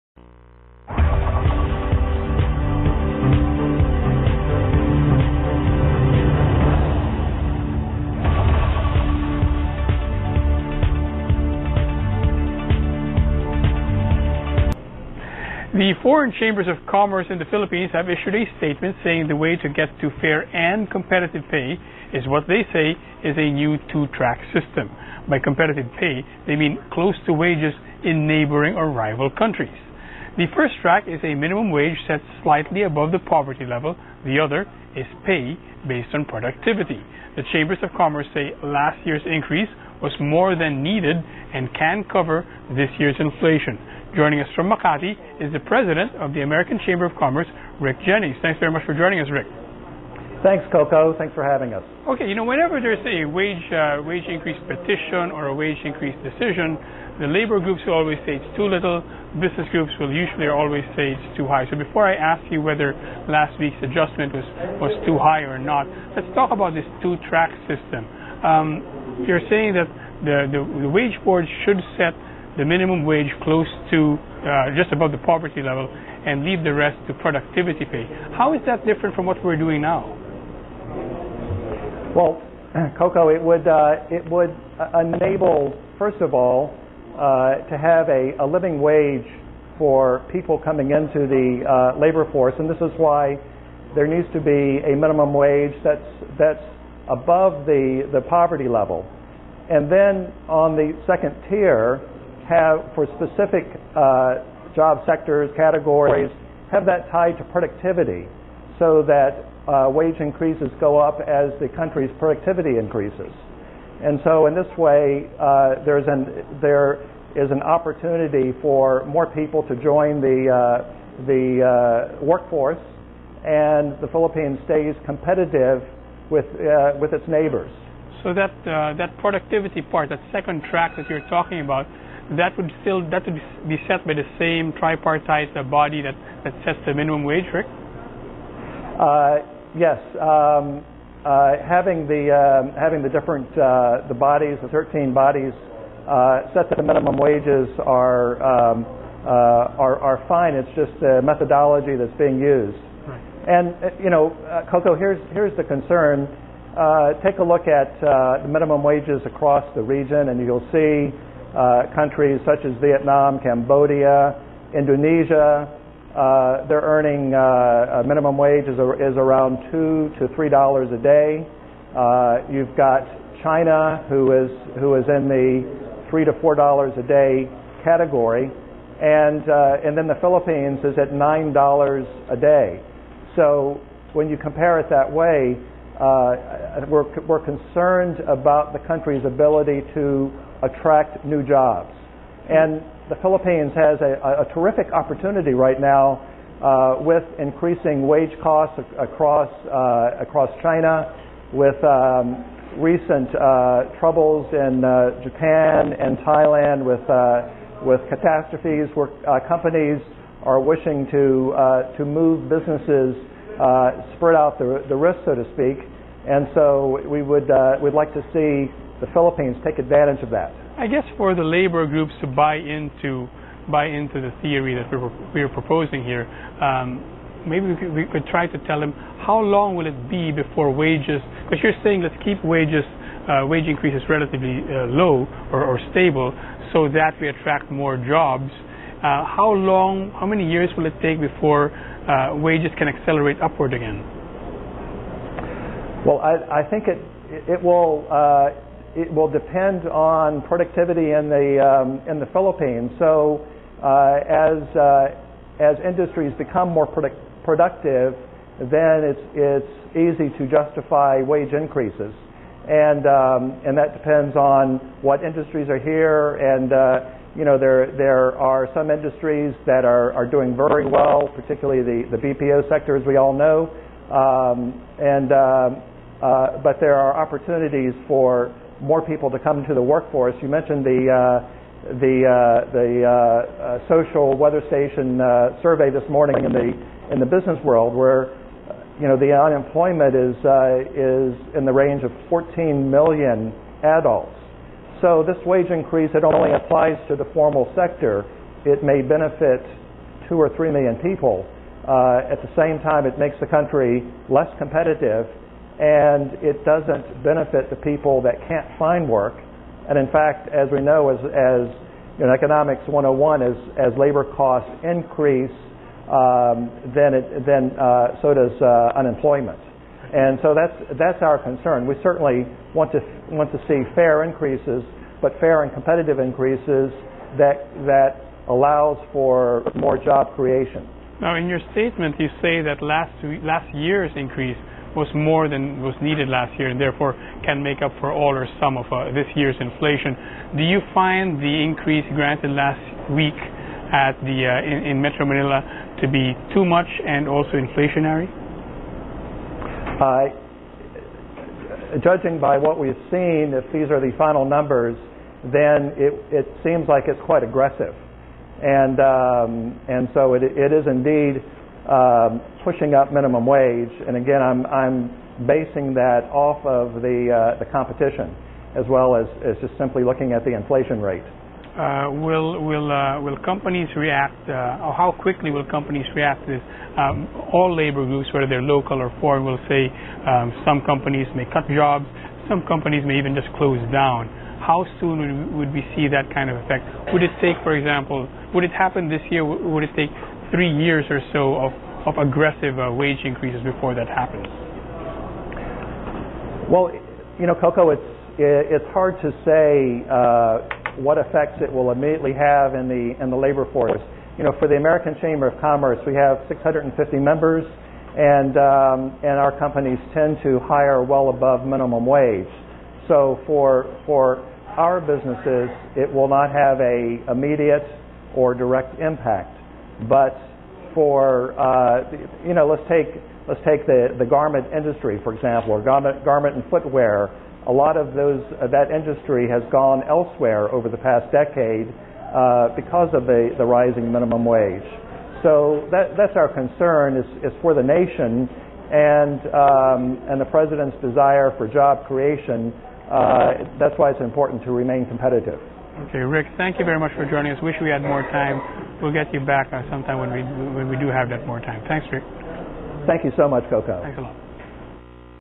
AmCham President Jennings interview with Alcuaz (ANC) on minimum wage hike
American Chamber of Commerce of the Philippines, Inc. President Rhicke Jennings interview with Coco Alcuaz on minimum wage hike (as seen on ANC)